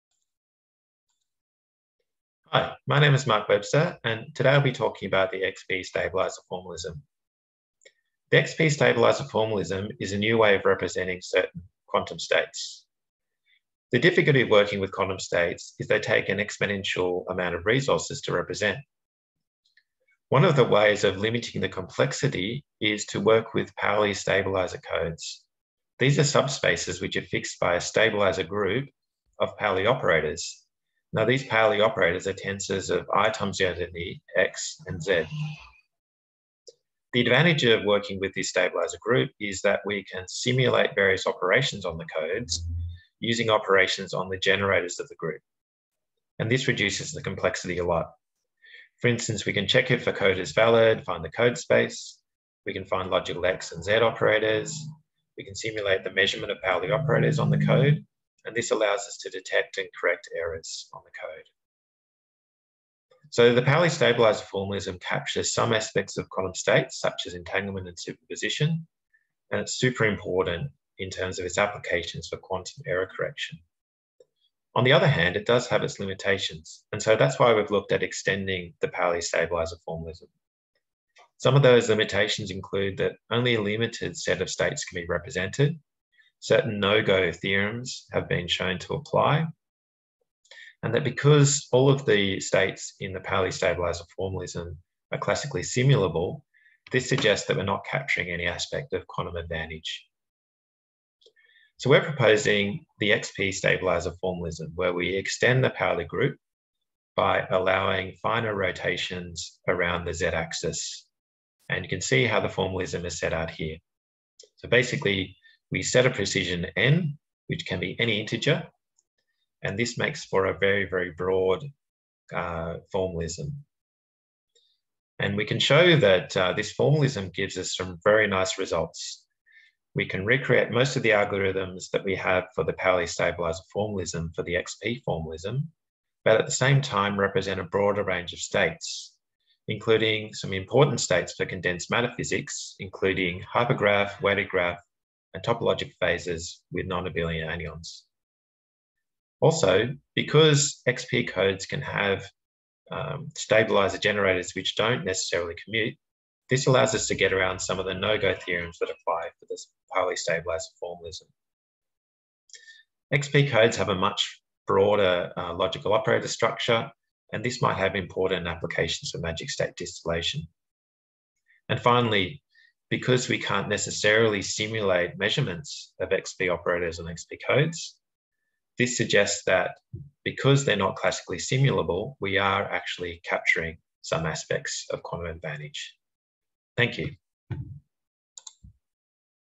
Introducing the XP Stabilizer Formalism
The XP Stabilizer is a new way of representing quantum states and is an extension to the Pauli Stabilizer Formalism.
We set out the new formalism and show how it allows us to represent a wider range of states than the Pauli Stabilizer Formalism.